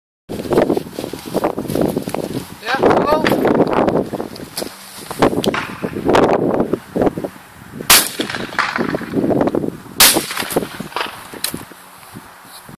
BAIKAL side by side shotgun sporting clay stand
Quick video of me testing my baikal side by side shotgun on sporting clay stand at fauxdegla